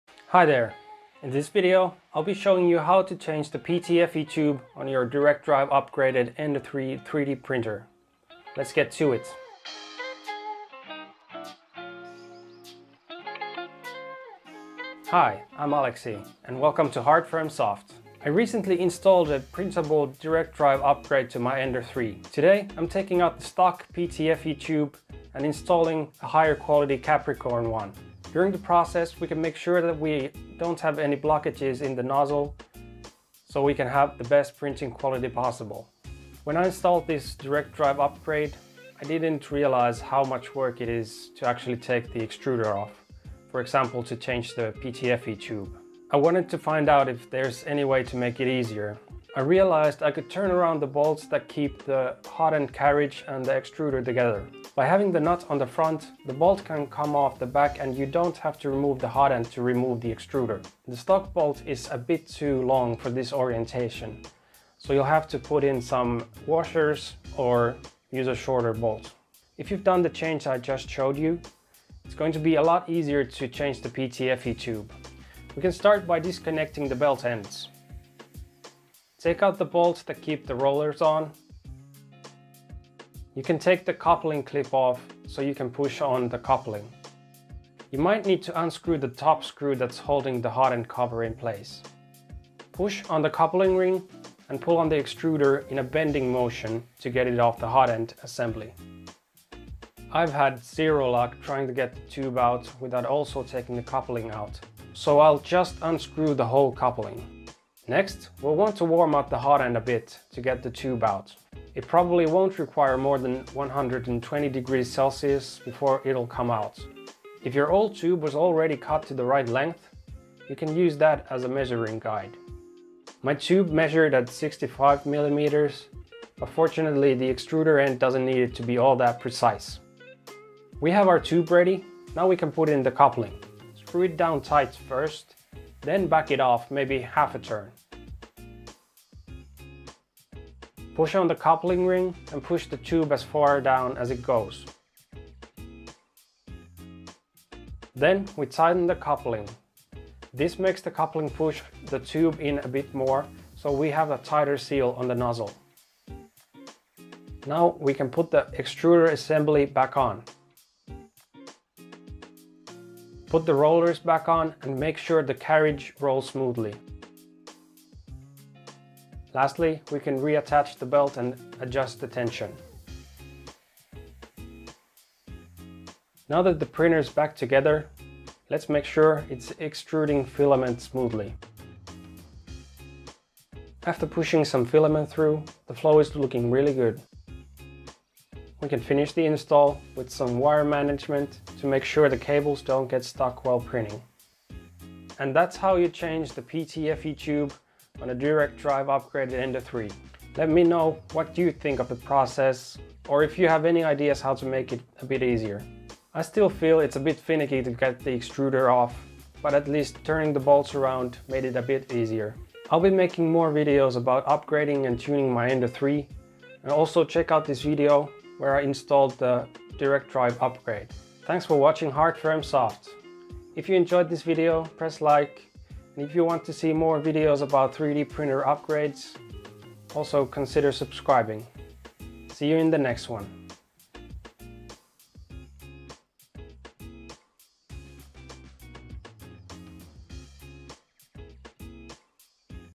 Change PTFE Tube for Ender 3 Direct Drive Upgrade
I'll show you how to change the PTFE tube on a direct drive Ender 3 3D printer. I installed this direct drive upgrade in a recent video which is linked below. I'm upgrading the stock tube that came with the printer to a higher quality Capricorn PTFE tube. PTFE tube replacement is a good way to improve your print quality and increase maximum print temperature. For the Ender 3 direct drive, PTFE tube replacement isn't quite as easy as it is on the original bowden setup.